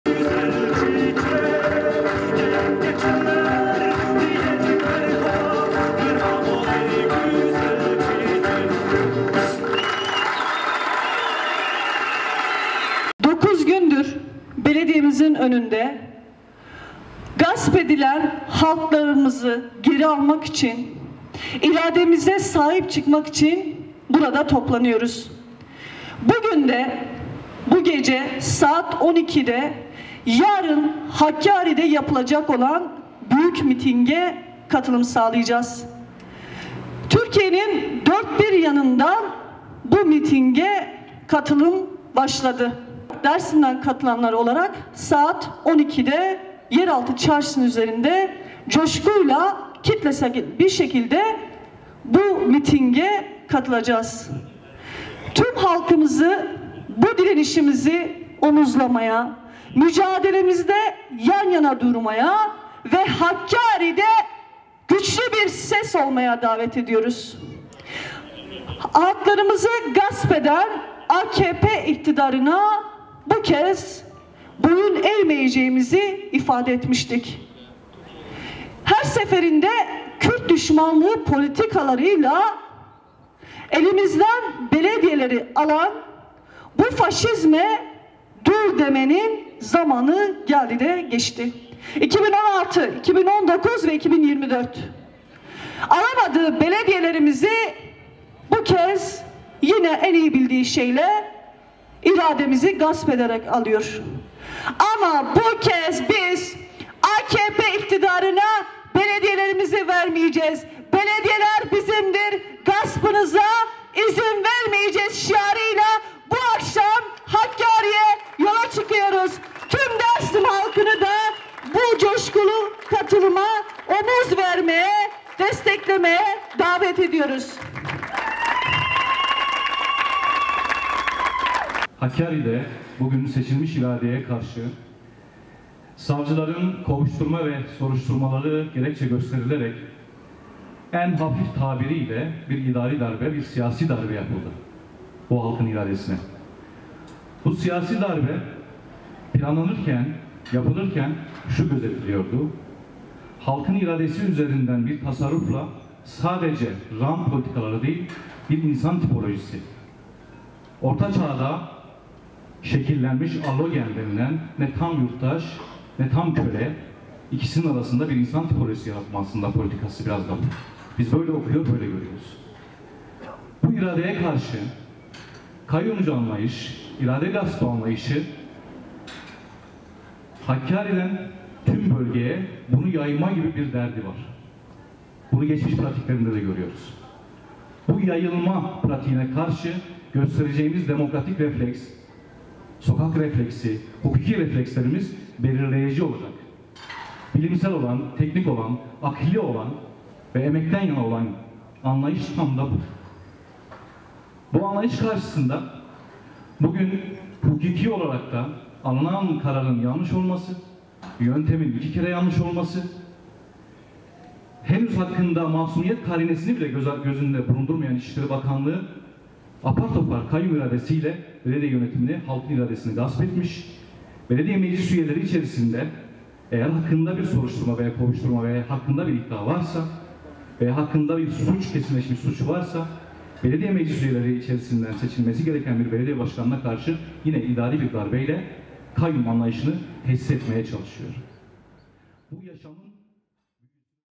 Dersim’de kayyıma tepki nöbeti 9. gününde: Hakkari'deki mitinge katılacağız
PİRHA-Hakkâri Belediyesi’ne kayyım atanmasına tepki göstermek için Dersim’deki nöbet eylemi 9. gününde devam etti. Gasp edilen iradelerini geri almak için 9 gündür direndiklerini söyleyen Dersim Belediyesi Eş Başkanı Birsen Orhan, “Yarın Hakkâri’de yapılacak mitinge biz de Dersim’den coşkulu bir katılacağız" dedi.